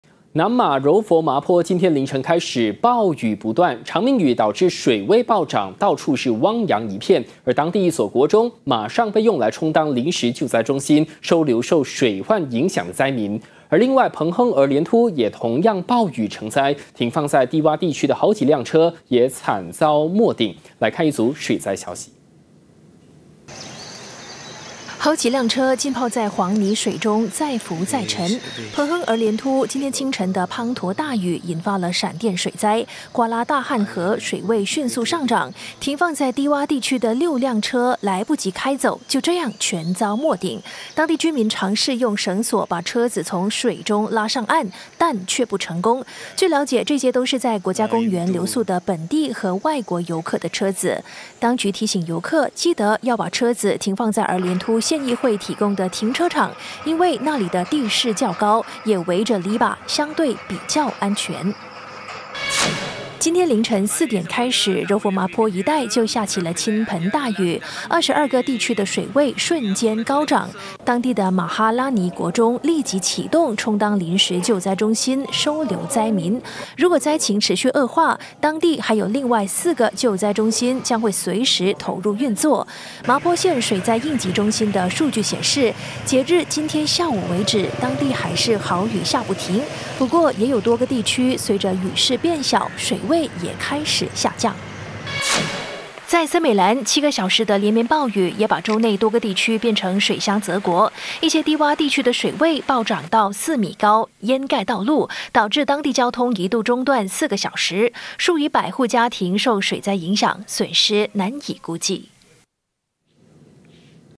西马多地凌晨降暴雨 麻坡.而连突一雨成灾
南马柔佛麻坡今天凌晨开始暴雨不断，长命雨导致水位暴涨，到处汪洋一片。当地一所国中马上被用来充当临时救灾中心，收留受水患影响的灾民。另外，彭亨而连突也同样暴雨成灾，停放在低洼地区的好几辆车也惨遭没顶。